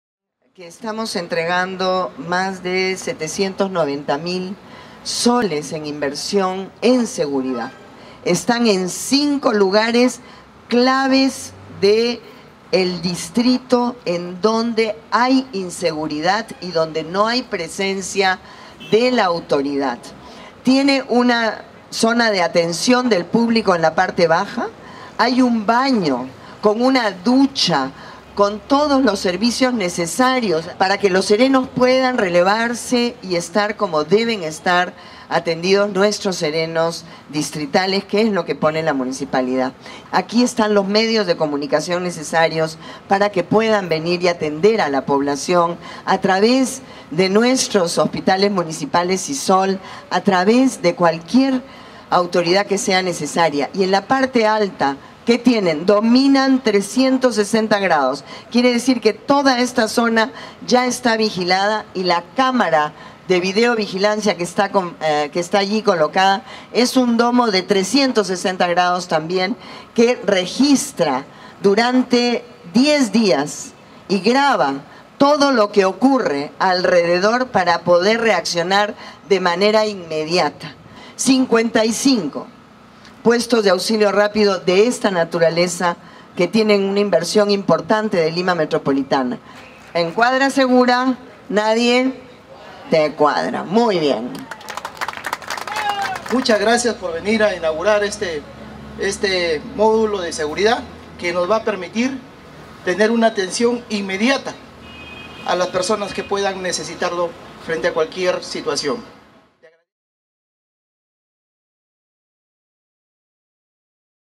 Alcaldesa Susana Villarán entregó cinco Puestos de Auxilio Rápido Metropolitano a Villa El Salvador
La alcaldesa de Lima, Susana Villarán, entregó cinco Puestos de Auxilio Rápido Metropolitano - Parmet a la Municipalidad de Villa El Salvador, con el objetivo de reforzar la vigilancia en ese distrito y seguir contribuyendo al desarrollo integral en materia de seguridad ciudadana mediante una política que abarca a los 43 distritos de la capital.
Con una inversión de 769 mil 980 nuevos soles, la Municipalidad Metropolitana de Lima se encargó de construir e implementar estos puestos de seguridad, que cuentan con un monitor con cámara de videovigilancia que funcionará durante 24 horas del día, así como escritorios, sillas, computadoras, kit de primeros auxilios y teléfono.